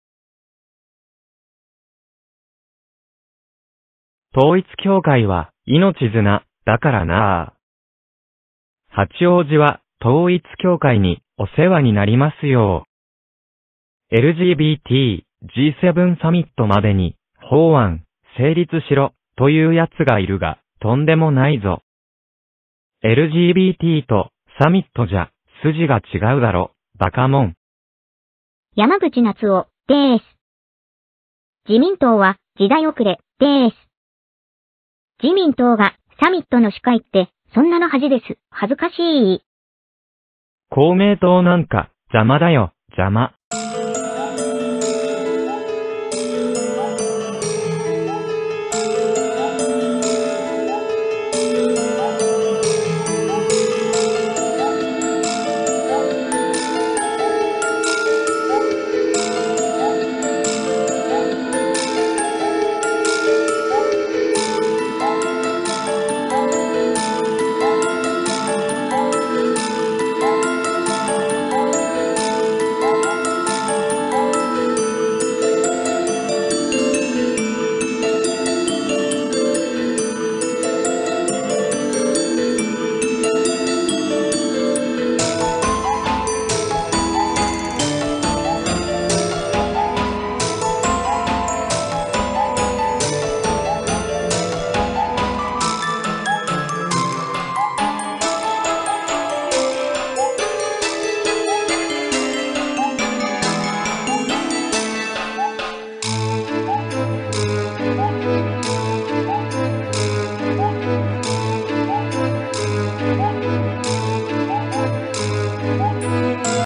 萩生と統一教会は,八王子市民を誤魔化し,次にＬＢＧＴ法もみ消そうと。山口公明党と亀裂？
23/4/26　八王子市議選は2名の落選被害。